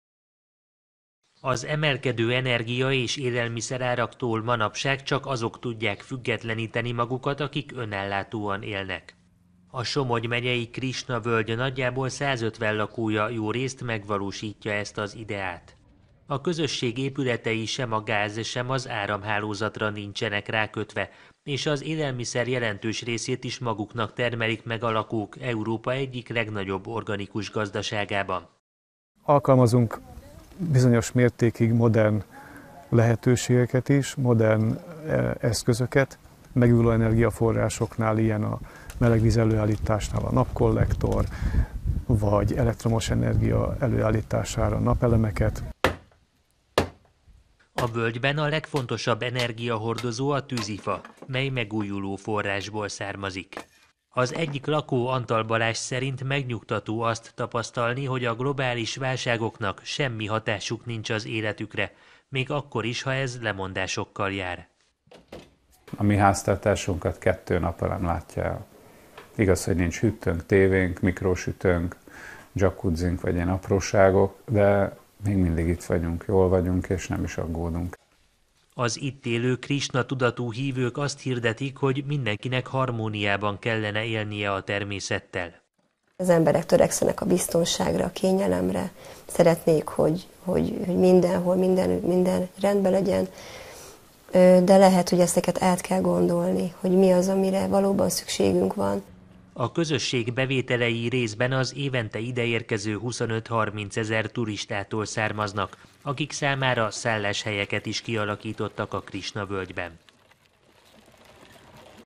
Sem a gázáremelés, sem az infláció nem számít a Krisna-völgy lakóinak
A nagyjából 150 fős Somogy megyei közösség házai nincsenek rákötve az országos áram- és gázhálózatra, ami válságos időkben nagyon jól jön.